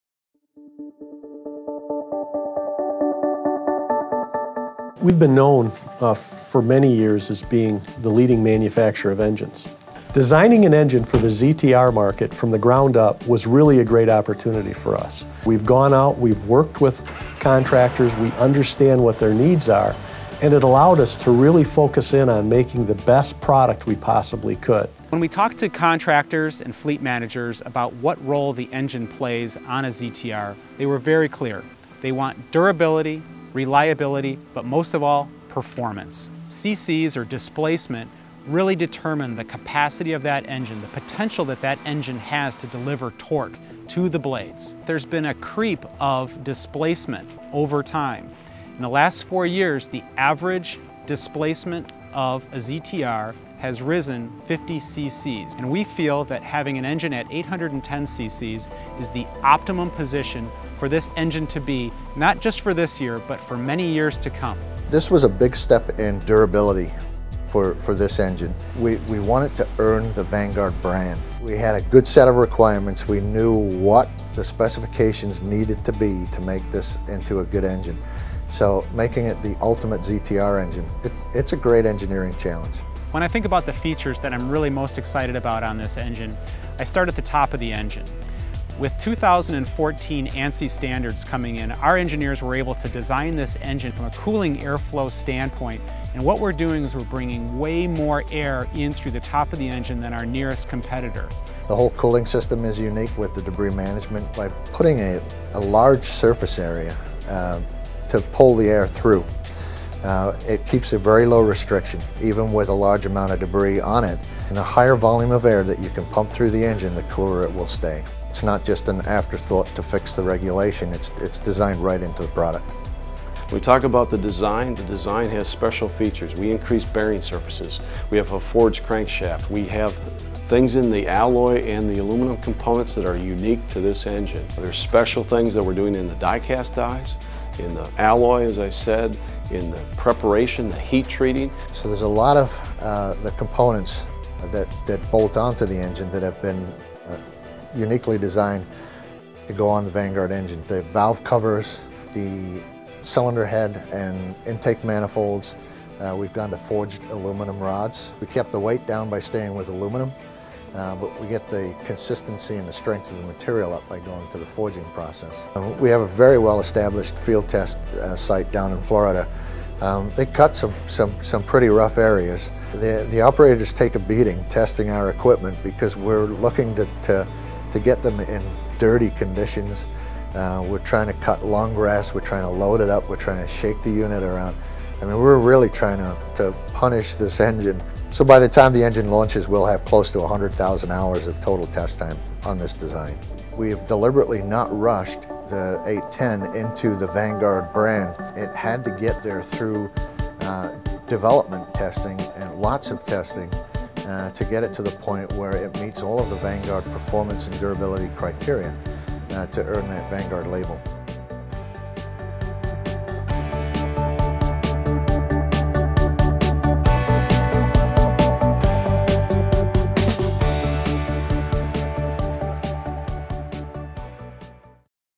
Vanguard 810 Optimum: Parts & Testing
At an optimum 810cc, Vanguard's new line of commercial-grade engines establishes a new standard for zero-turn engine displacement. That means a more efficient, more powerful and, overall, more profitable commercial zero-turn mower. With better air management, lower vibration, and over 100 unique durability-enhancing components, Vanguard's 810cc engines allow turf professionals to go farther, cut longer, and maximize productivity.